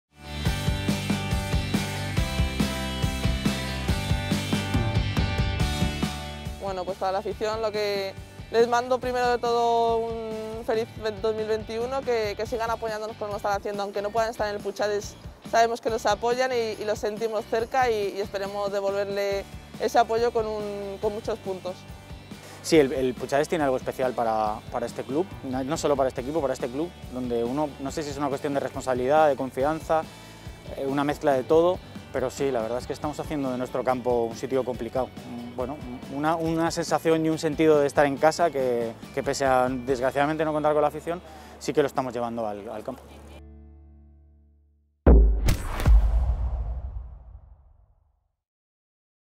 EL DÍA DE REYES NOS TRAE EL DERBI TEIKA⚽👑
El estadio Antonio Puchades de la Ciutat Esportiva VCF acoge a las 18.30h de este miércoles, 6 de enero de 2021, el derbi Teika entre el VCF Femenino y el LUD Femenino. El partido, correspondiente a la jornada 14 de la Primera Iberdrola 2020.21 va a ser televisado en directo por Gol y retransmitido por VCF Media Radio.

The Antonio Puchades will host this Wednesday 6th of January 2021, the TEIKA derby between Valencia CF Femenino and LUD Femenino on matchday 14 of Primera Ibedrola which will be televised on GOL and broadcasted by VCF Media Radio.